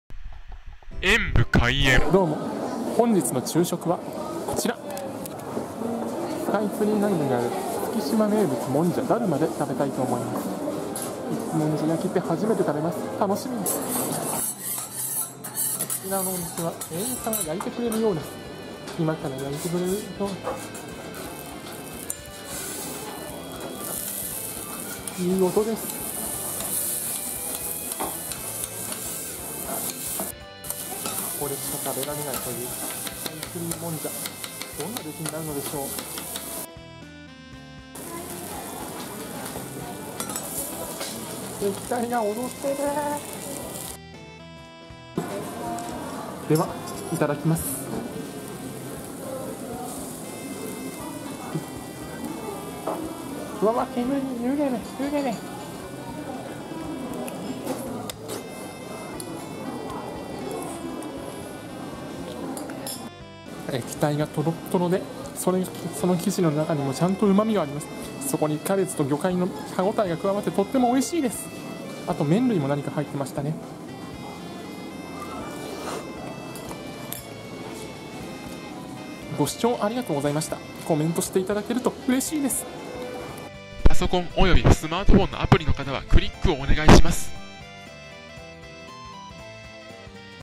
東京弾丸ツアー 昼食に月島名物だるまのもんじゃ焼きを食べてみた もんじゃ焼き、初めて！！
東京弾丸ツアーの昼食はこちら！！
月島名物だるまのもんじゃ焼きです。もんじゃ焼き初めて、そのお味は～